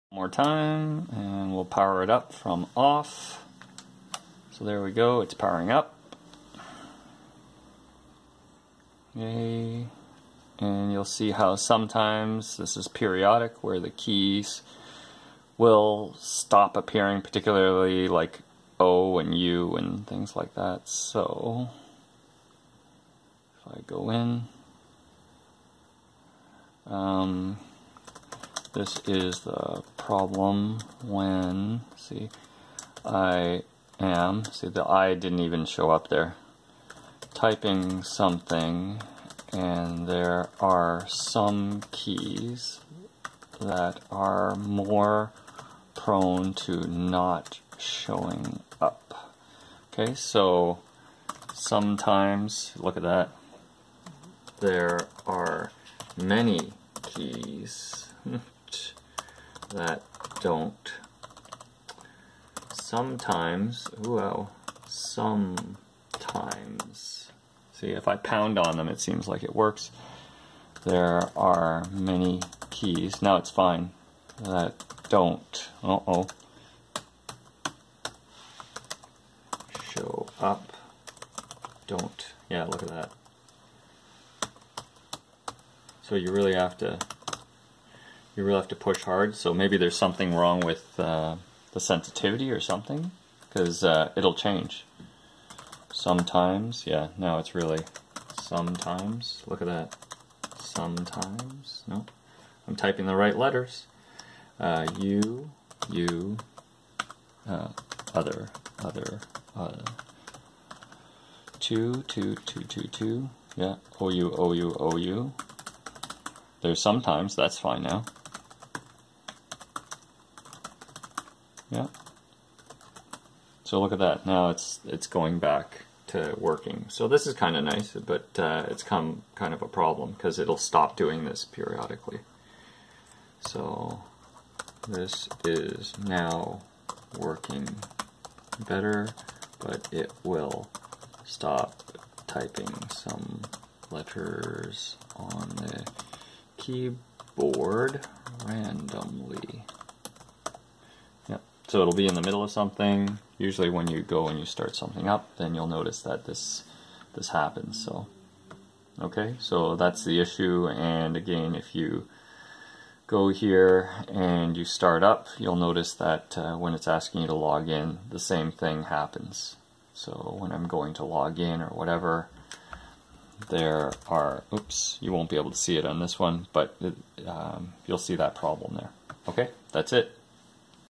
Asus C300S Chromebook keyboard problem * NOT A FIX * = try actually reading for a change
*NOT A FIX* - Created this video for our warranty submission to Asus. This Chromebook has caused us a couple of problems. There was an issue with the battery connection I believe in side and the Chromebook shutting down on its own when grabbing the sides of the Chromebook beside the touchpad. This time it's a problem with some letters not appearing when you're typing on the keys.  Particularly o, e, p, i, q come to mind.